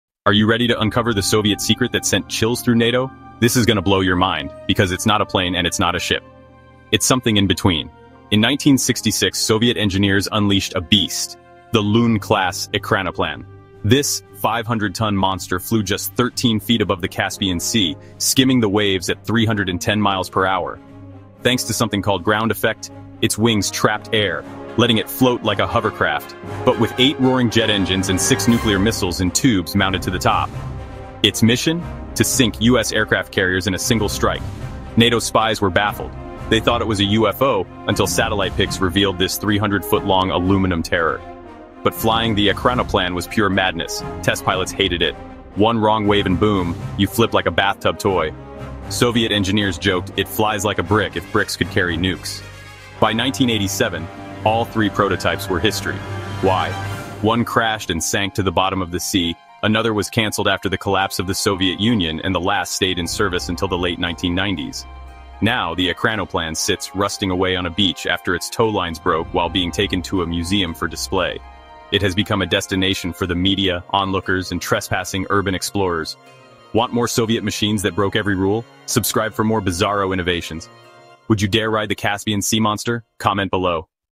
The Caspian Sea Monster: The USSR’s 500-Ton Flying Ship
In 1966, the Soviet Union spent $7 billion to build a nuclear-armed ‘flying ship’ that raced across oceans at 300 mph—then abandoned it. Why did this engineering marvel become a rusting relic?